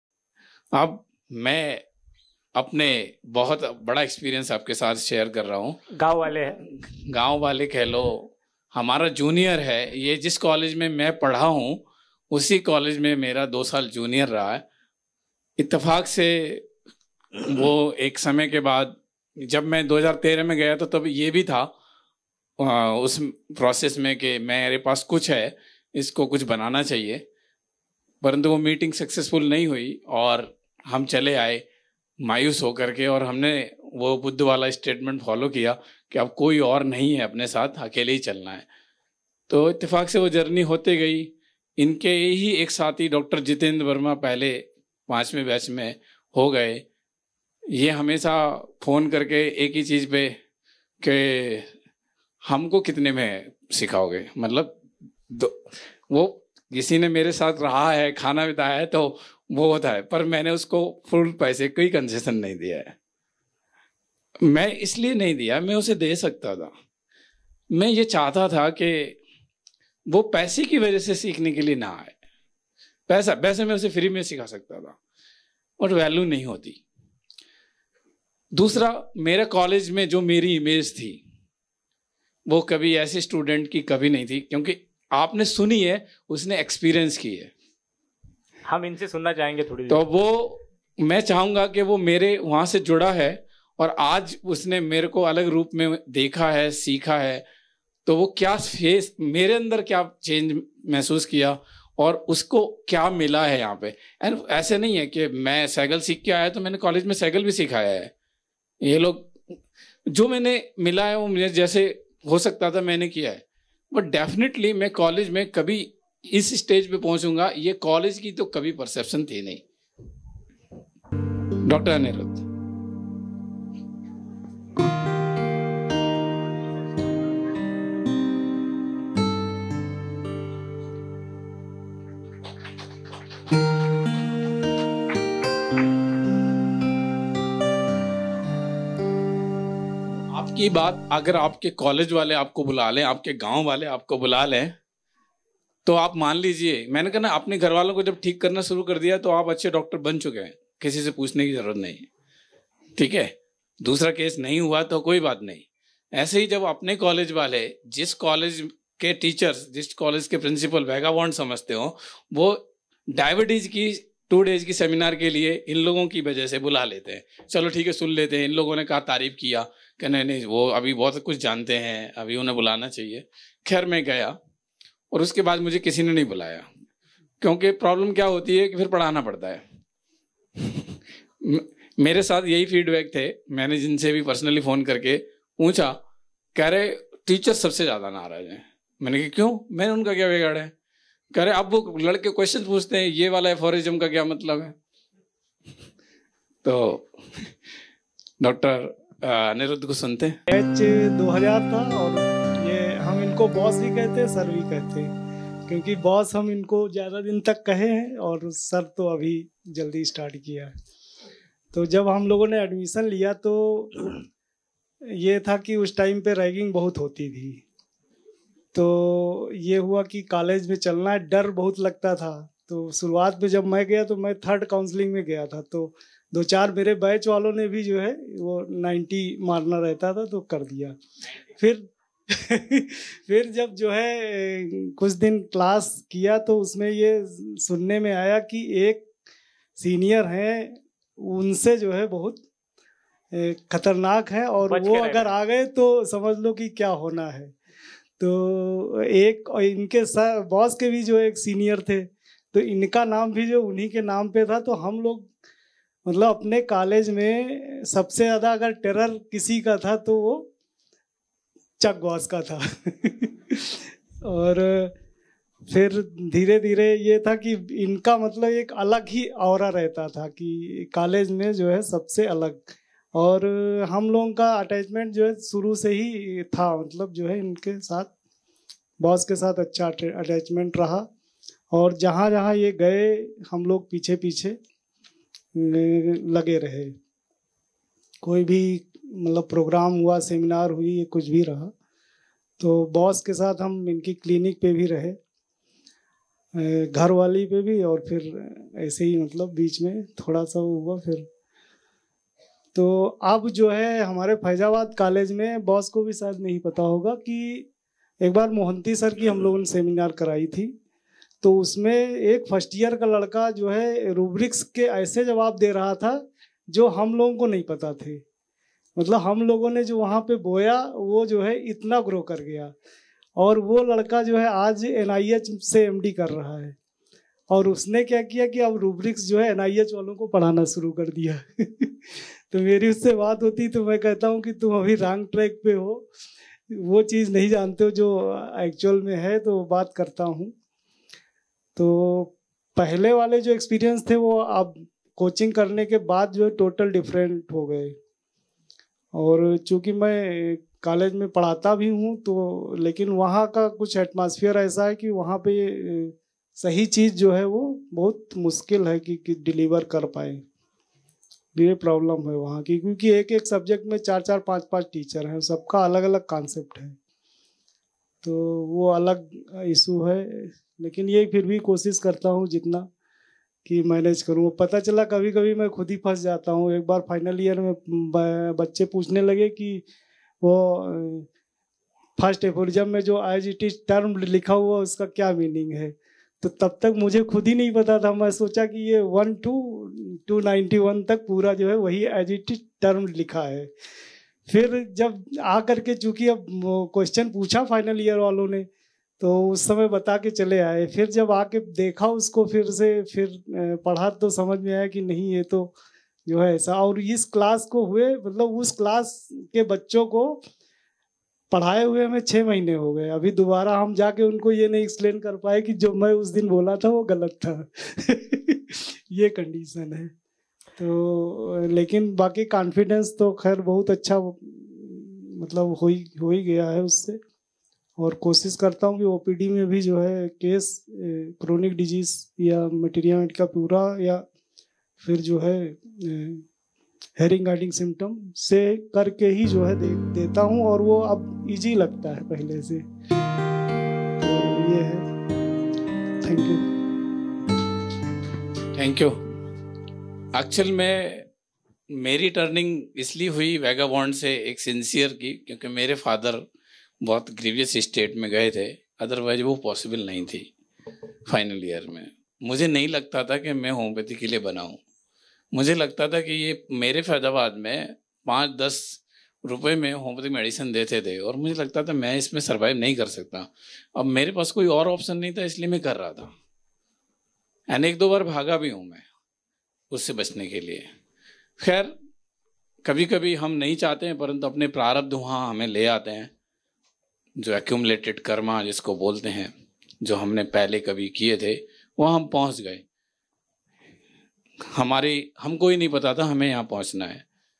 Testimonial - Dr Anirudh Verma - VHSC Alumni - about VHSC Training
The Practitioner of Homeopathy of various methods (and Reader at National Homeopathic Medical College from Lucknow, Uttar Pradesh - referred by Dr Jitendra Verma - VHSC Alumni - The Professor in Gorakhpur Homeopathic Medical College - from Faizabad, Uttar Pradesh) converted Hahnemannian Homeopathy sharing his experience about VHSC Training at Workshop on Virgin Homeopathy Scientific Coaching, VirHom Software and VHSC Certificate Convocation Ceremony on occasion of 267th Birthday (10th April 2022) of the Master Samuel Hahnemann